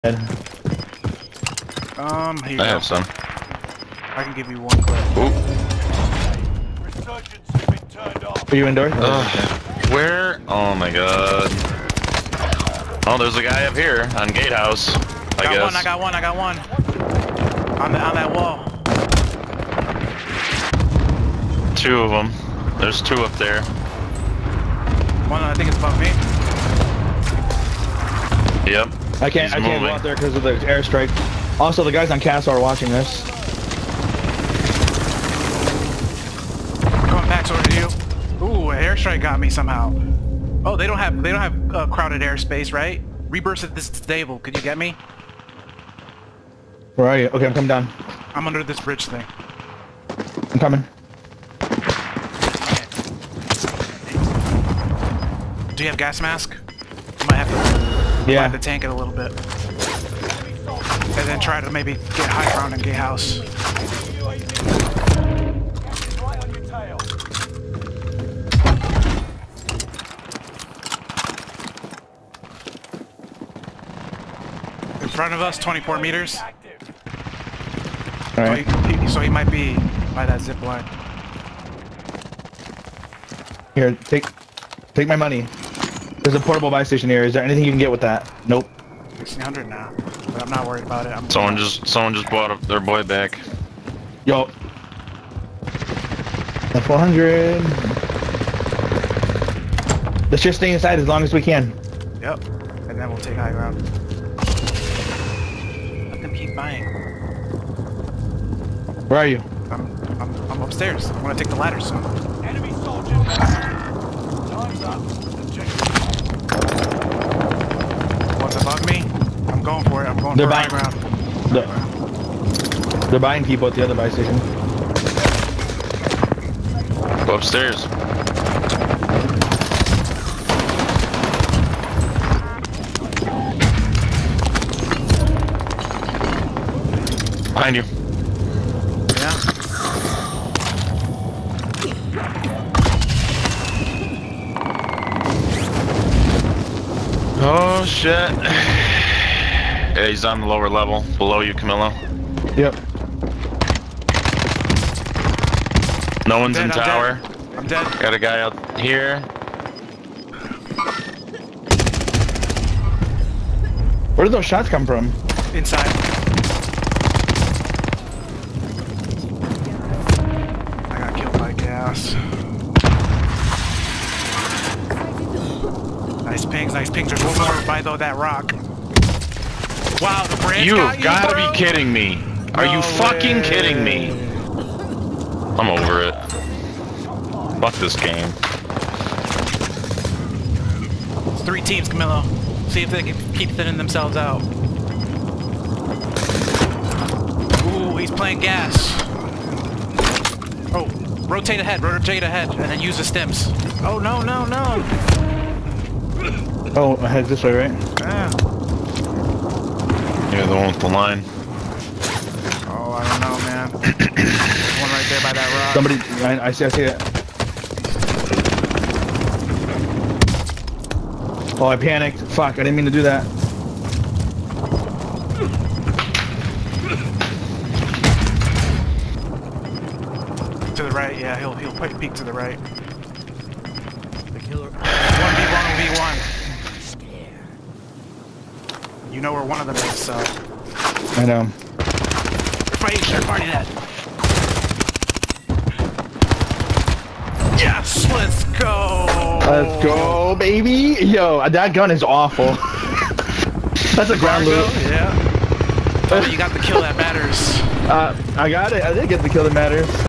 Fortunes Win | Hectic Endgame & Gas plays
Final circles approach, there are still 5 teams alive. It's only a matter of time before all hell breaks loose...